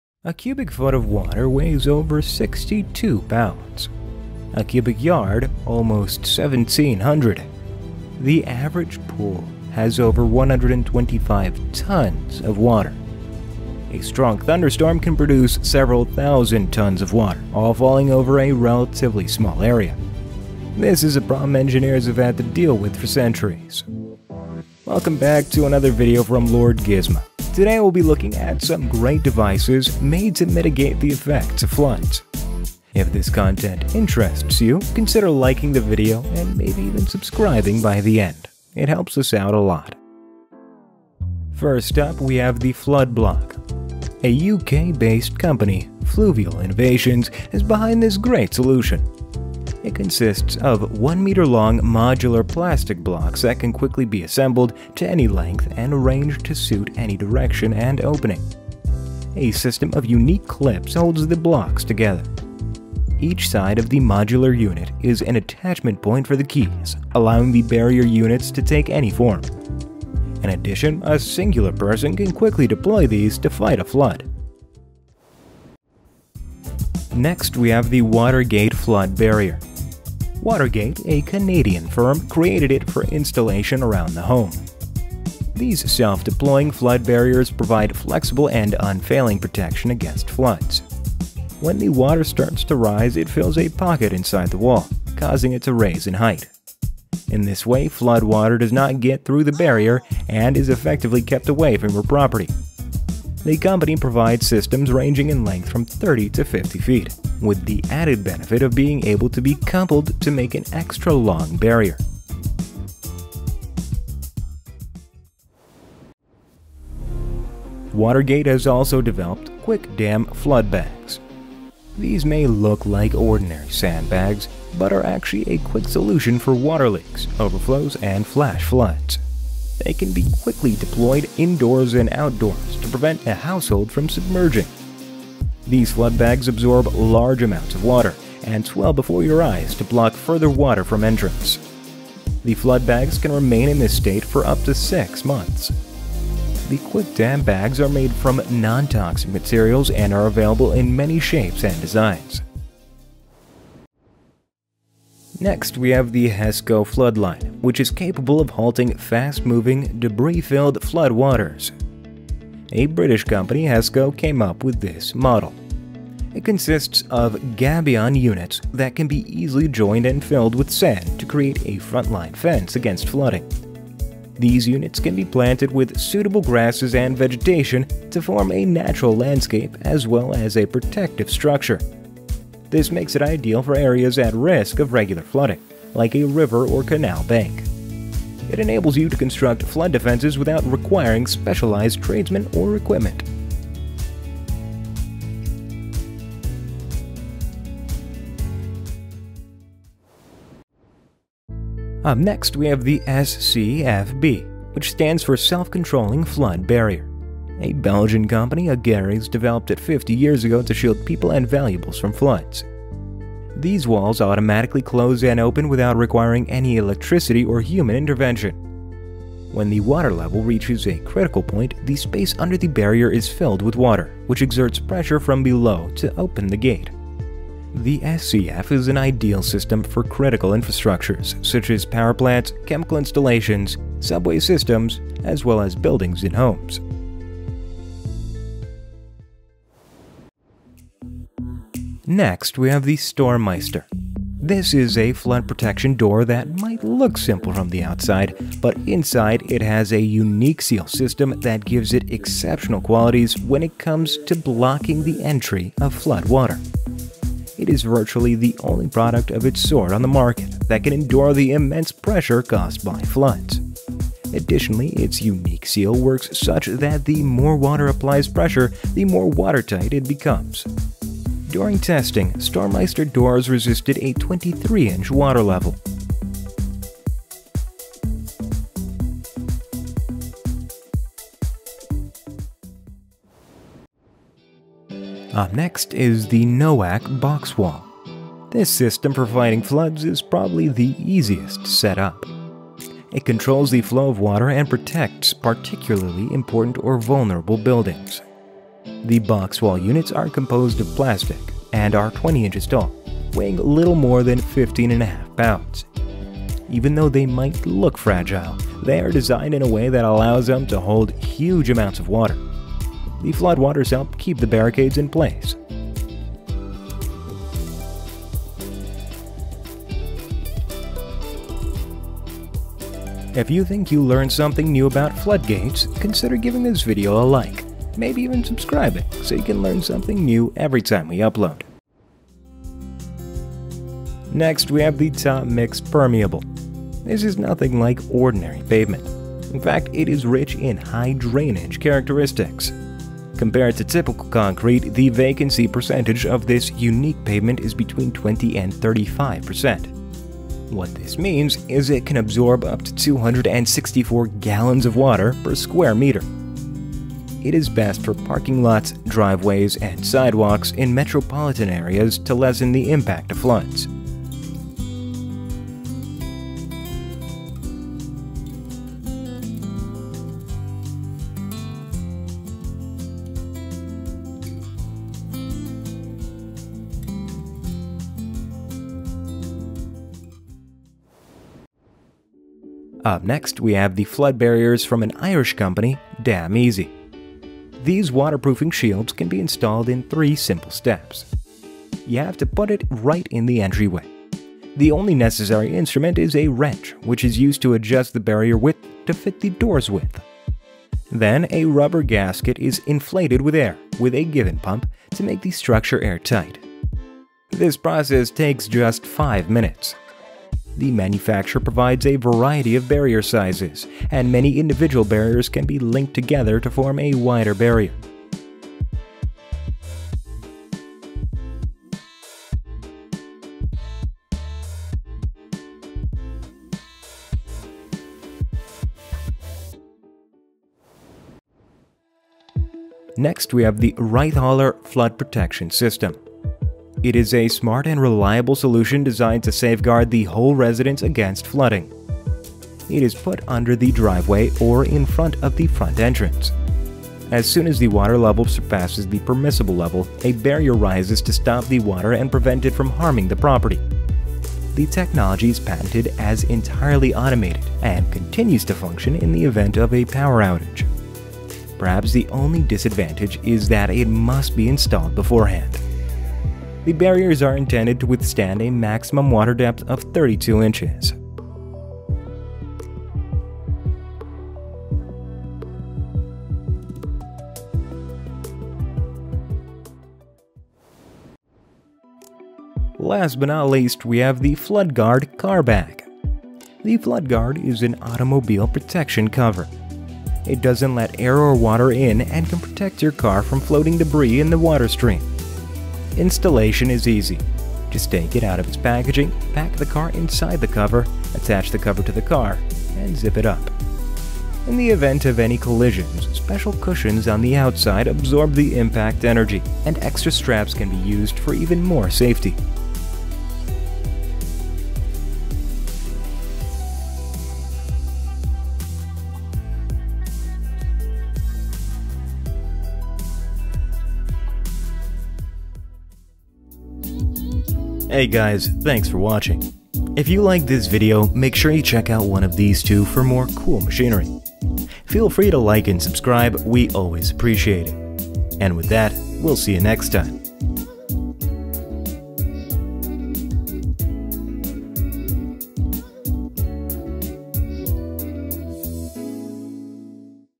You Gotta See These Anti-Flood Inventions That Are At Another Level
A cubic foot of water weighs over 62 pounds. A cubic yard, almost 1,700.  The average pool has over 125 tons of water. A strong thunderstorm can produce several thousand tons of water, all falling over a relatively small area; a problem engineers have had to deal with for centuries.  Welcome back to another video from Lord Gizmo!  Today we’ll look at some great devices made to mitigate the effects of floods.

00:00 INTRO

00:39 FLOODBLOCK™ BARRIER | FLUVIAL INNOVATIONS

01:15 WATER-GATE DAM

01:56 QUICK DAM

02:33 HESCO FLOODLINE

03:20 AGGERES SCFB

04:00 STORM MEISTER

04:44 NOAQ BOXWALL

05:31 TOPMIX PERMEABLE

06:21 DAM EASY® FLOOD BARRIER

07:11 FLOOD PROTECTION REITTHALER

08:01 FLOOD GUARD CAR BAG

08:46 Thanks for watching! Feel free to like and subscribe, we always appreciate it.